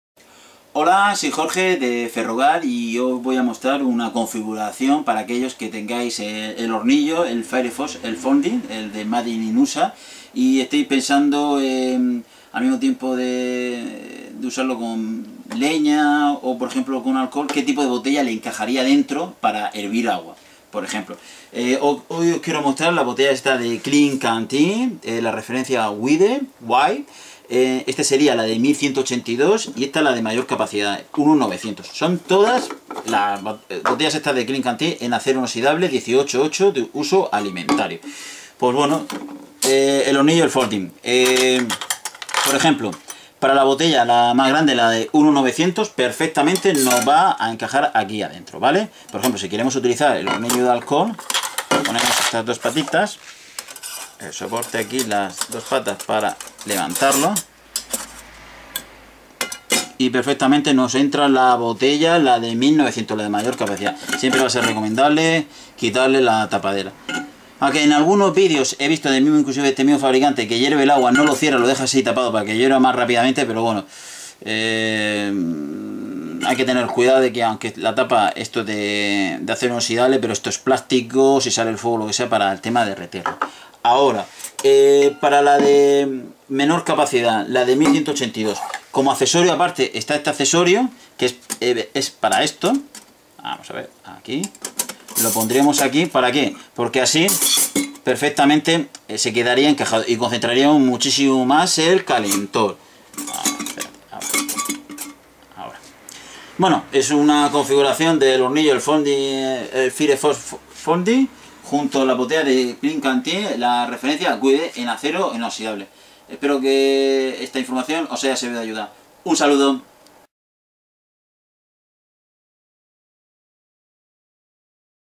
Klean Kanteen Wide boca ancha encaja en Firebox Folding
Siempre tenga mucho cuidado al calentar agua en una botella de acero, que se convertirá en extremadamente caliente y no deje la tapa en su lugar.
Al calentar debido a la presión causada por la expansión, podría causar una explosión.
Hornillo “Firebox Folding Stove” Acero Inox:
Klean Kanteen "Wide" 1900ml Acero Inoxidable: